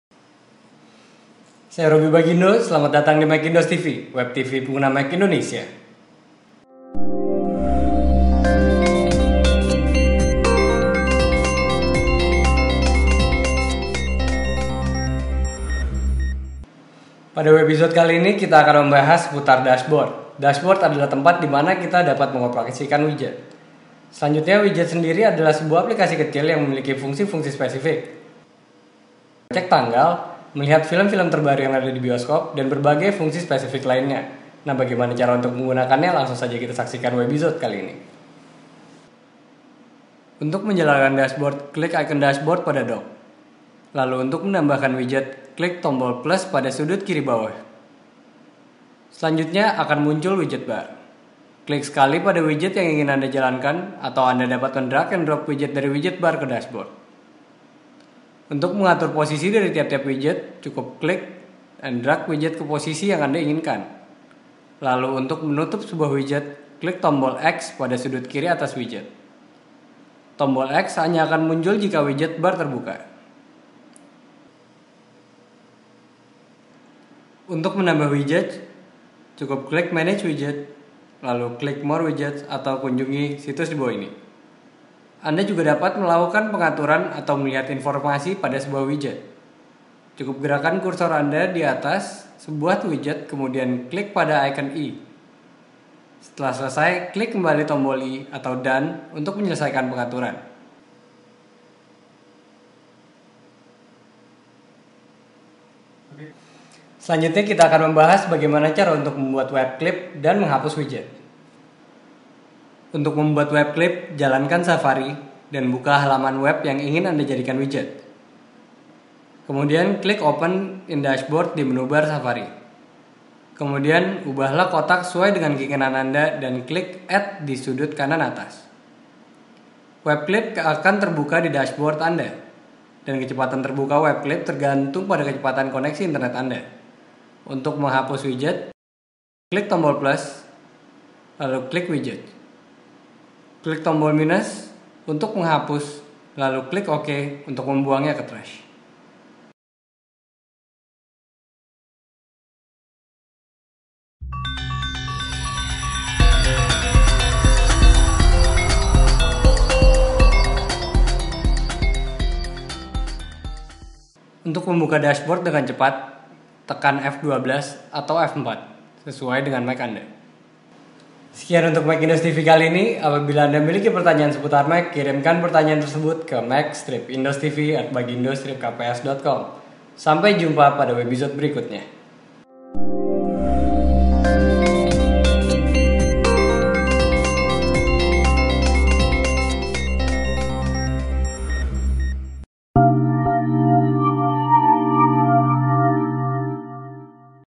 #050 Macindos.TV [MacDas] Ep #28 - Dashboard
Video tutorial tentang fitur Dashboard pada Mac OS X Leopard.Berbahasa Indonesia.